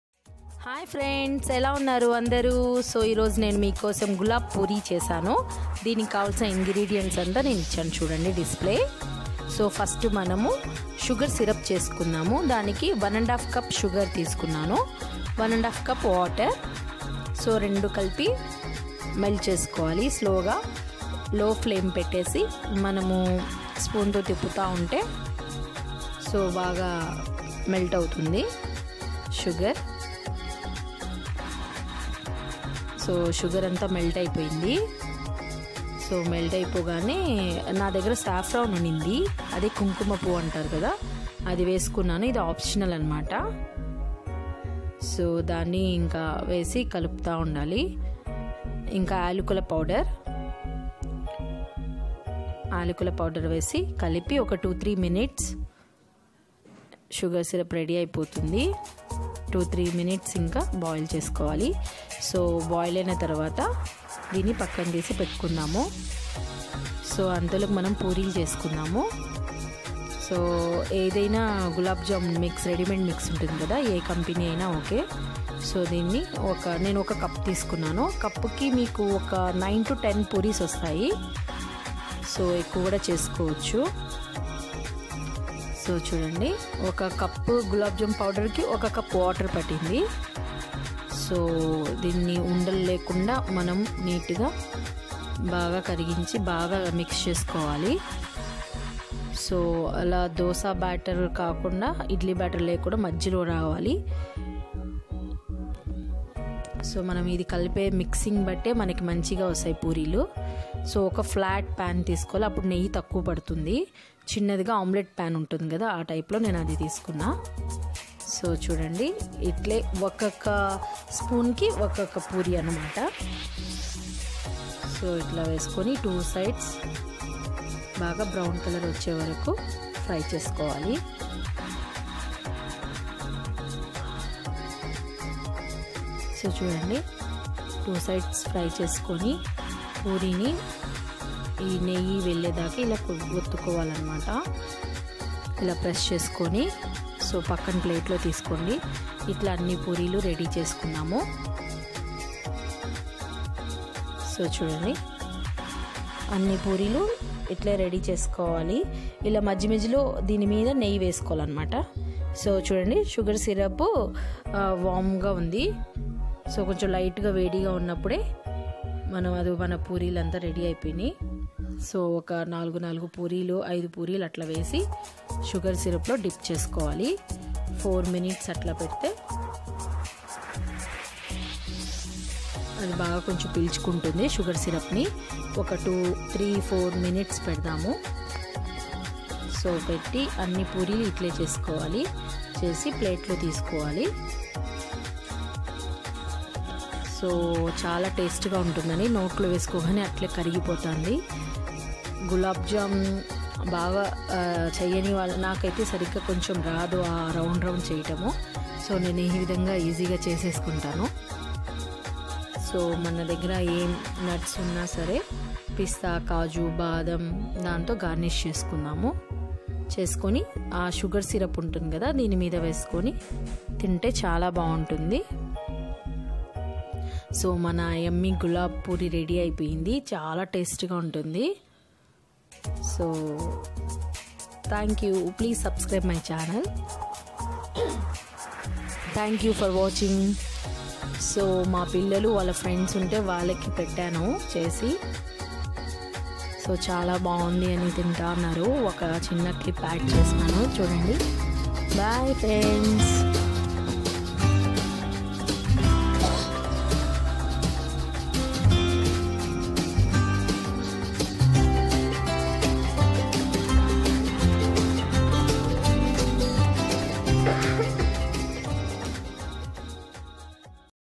😋😋 Gulab poori sweet 😋😋👍
Ingredients :
Gulab jamun powder - 1 cup
Sugar - 1.5
Water - 1.5
Cardamon powder - 1/2 tsp
Saffron stands - 10 to 15
Pista, caju, Badam for Garnish... Ready yummy Gulab poori 😋😋👍👌👌